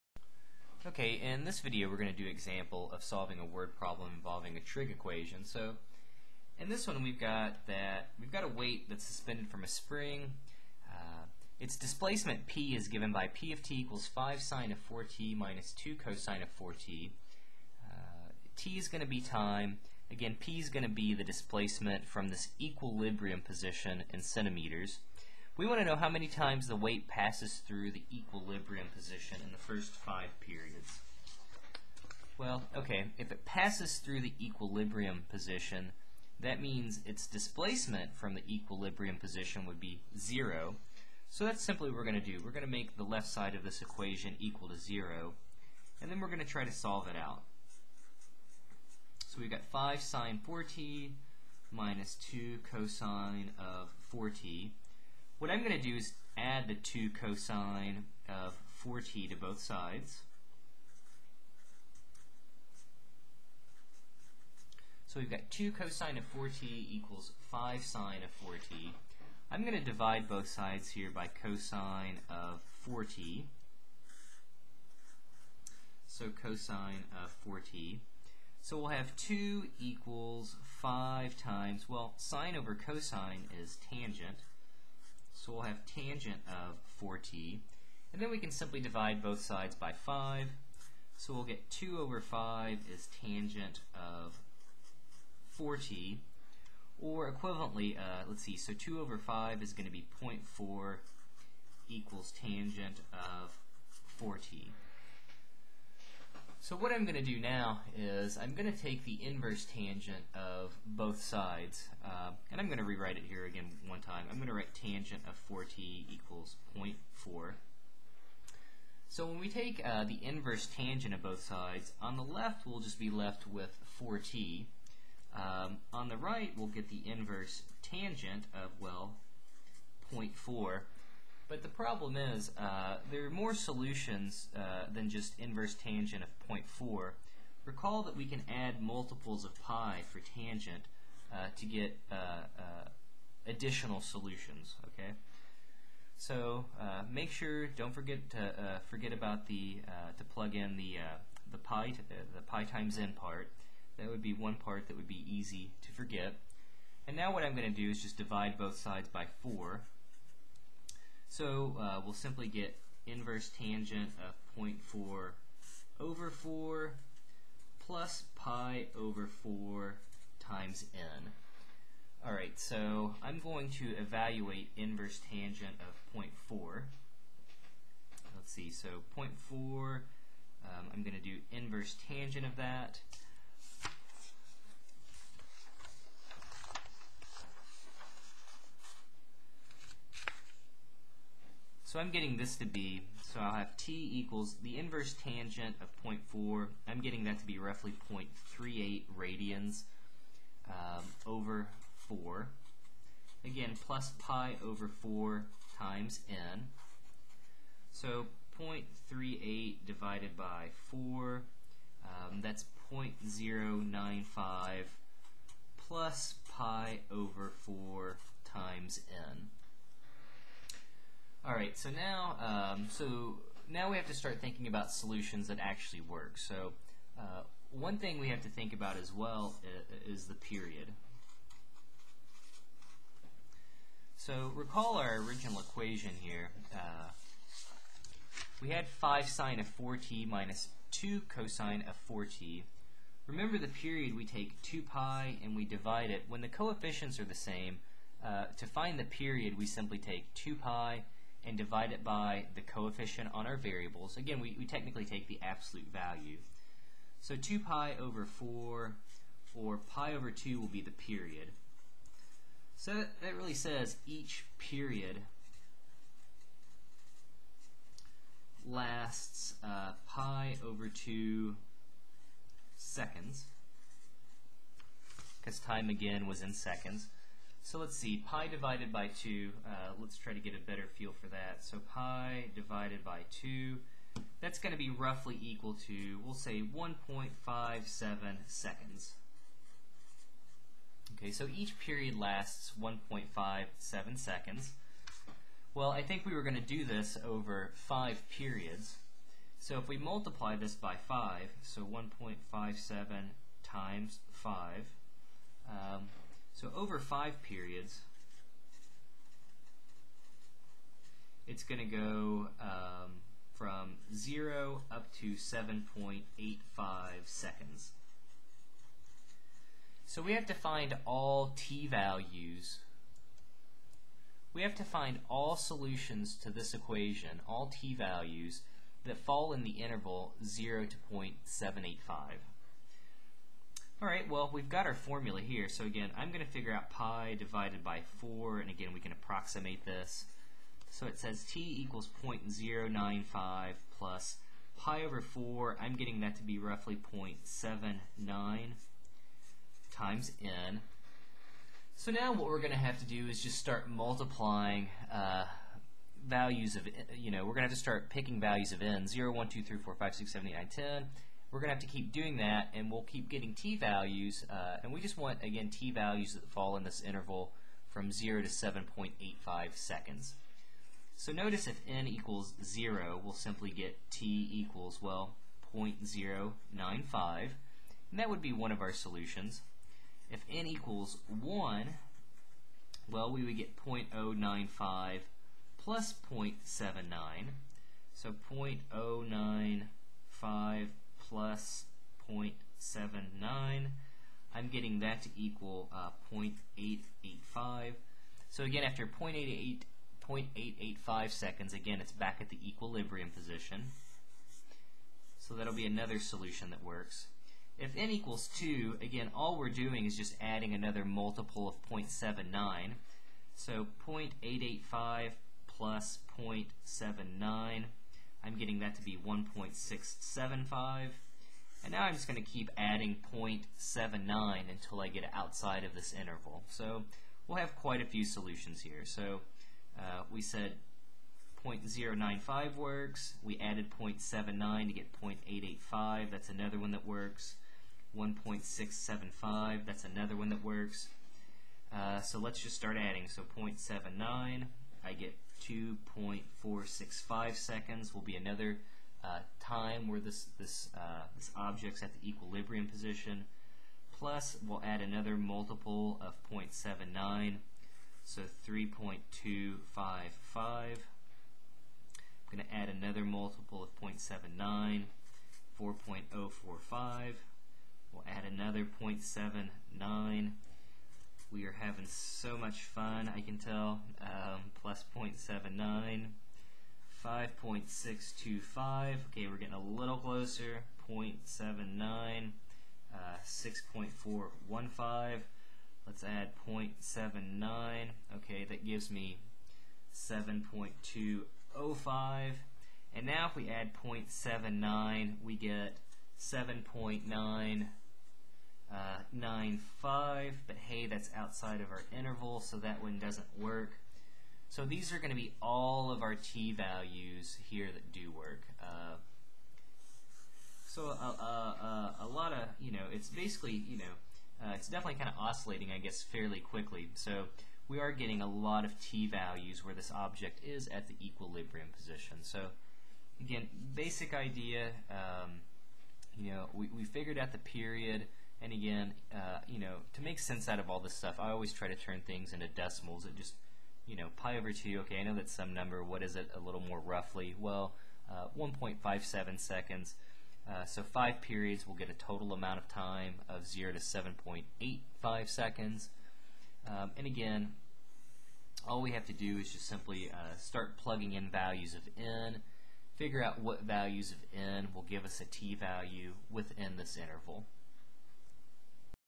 Solving Word Problems Involving Trigonometric Equations, Example 1
You da real mvps!   $1 per month helps!! Solving Word Problems Involving Trigonometric Equations, Example 1.  In this video I look at the equations describing the motion of a particular spring and answer some questions about it!